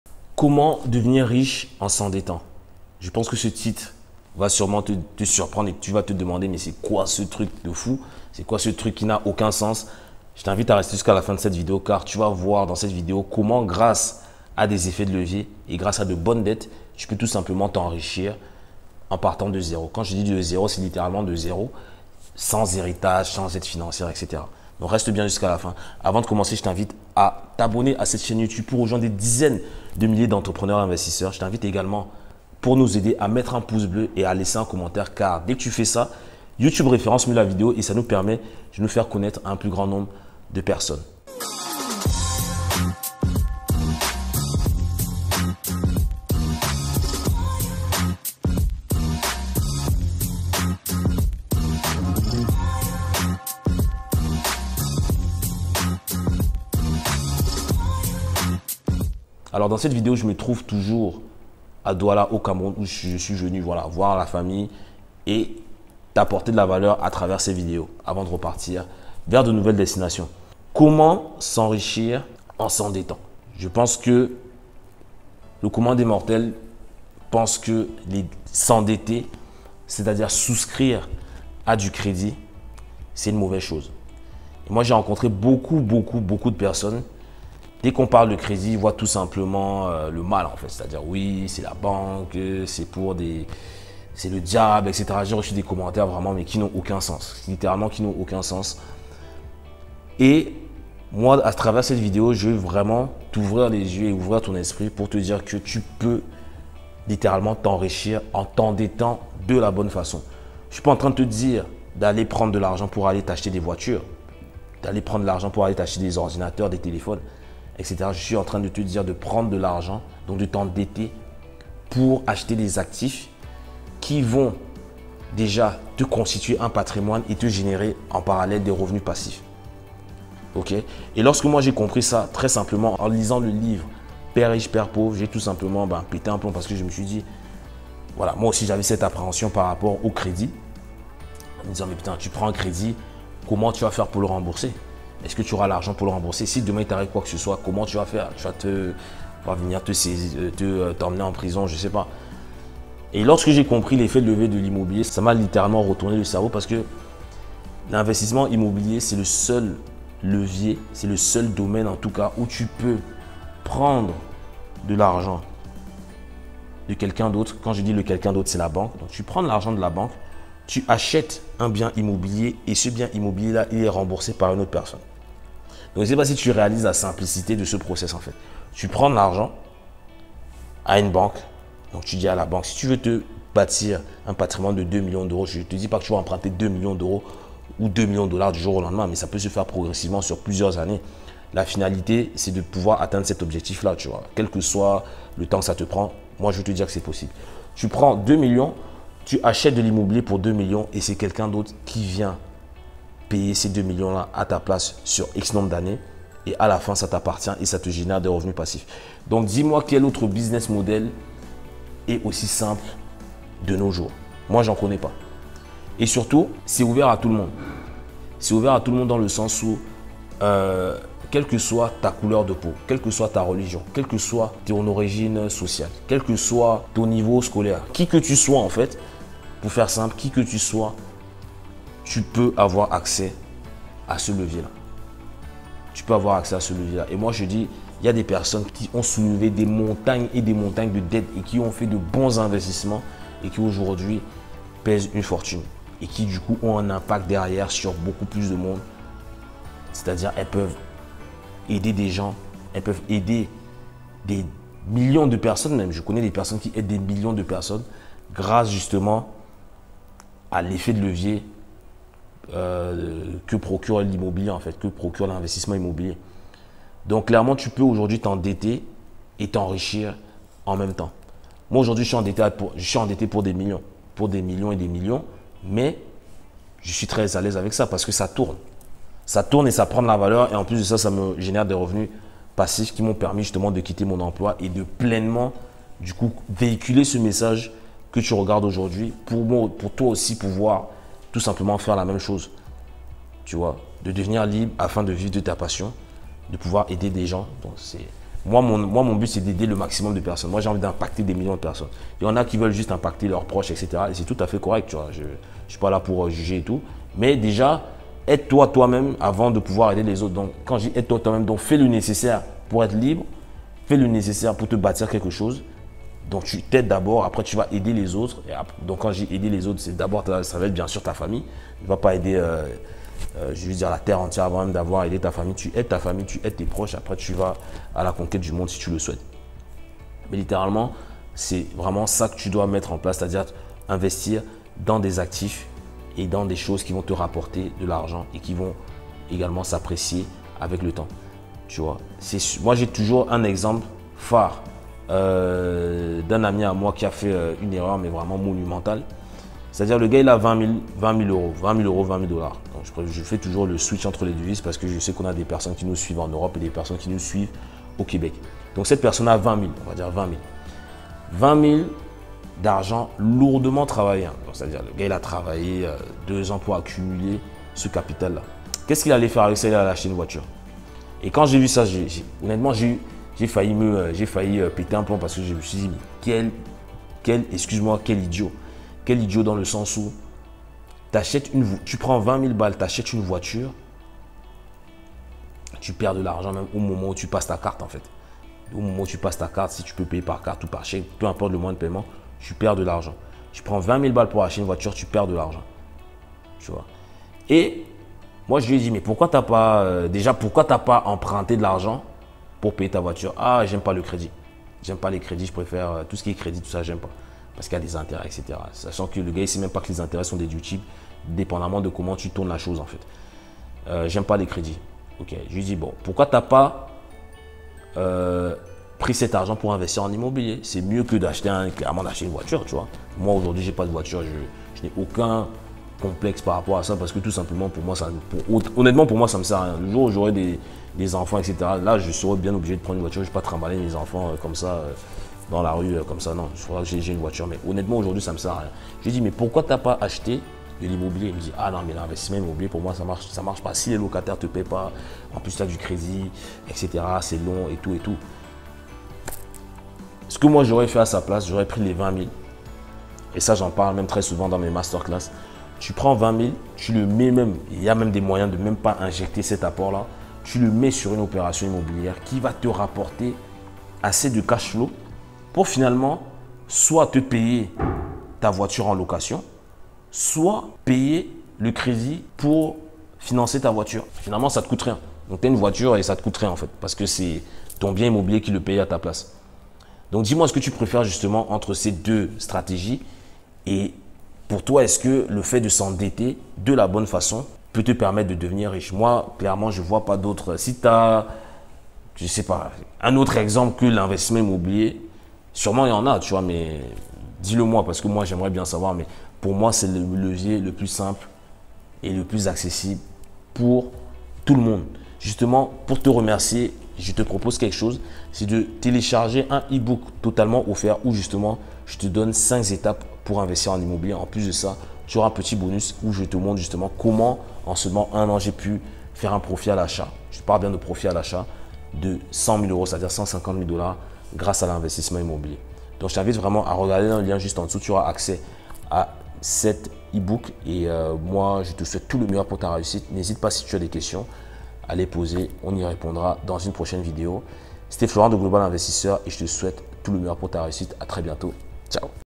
Comment devenir RICHE en s'endettant (20/30)
Comment devenir RICHE en s'endettant
Dans cette nouvelle vidéo, je vais sûrement te surprendre mais je t'invite à la suivre attentivement. Sais-tu qu'il y a plusieurs façons d'investir en immobilier pour atteindre la richesse ? La stratégie que je t'explique ici prend tout son sens car le but n'est pas de s'endetter pour faire faillite, mais bien pour s'enrichir dans le but de donner naissance à ses projets immobiliers.

Avertissement